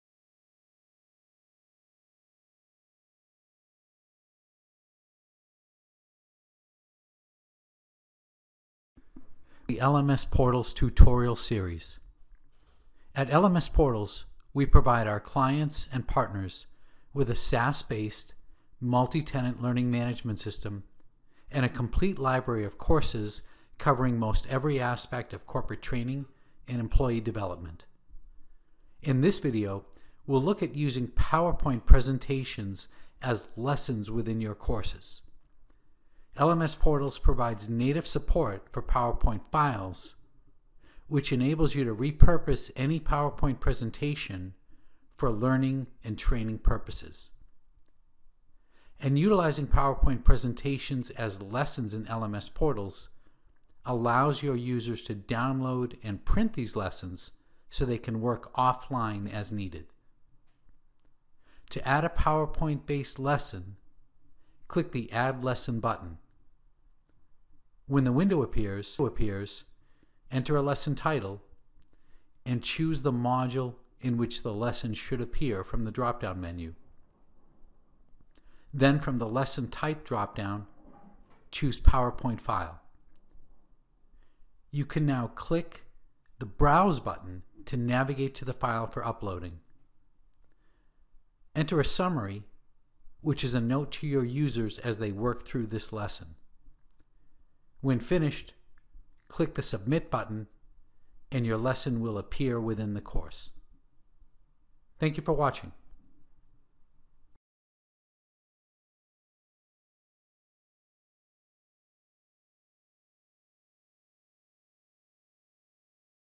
Adding a PowerPoint File as a Lesson in a Course
Hello everyone! In this video, we review how to add a powerpoint file as a lesson in a course.

LMS Portals is a SaaS-based, multi-tenant learning management system.  In this video, we look at the process for adding PowerPoint files as course lessons.

I hope you guys enjoy this video and find it useful.

➤Suggested Videos:-